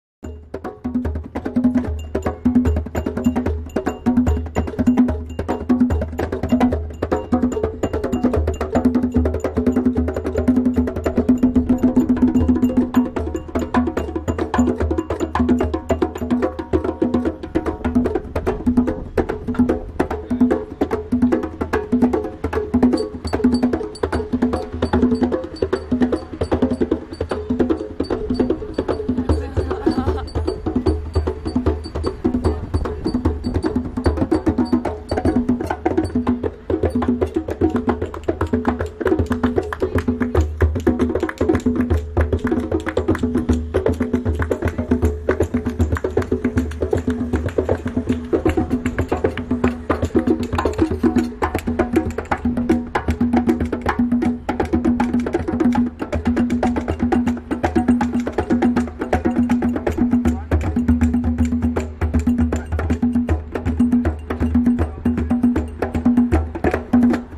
Red Zebra's Oli & Tom jamming in Bremen!
during the Heart Connection Tour training with the German team in Bremen, Oli & Tom were jamming away on the drums druing lunch-break at the Cafe Concordia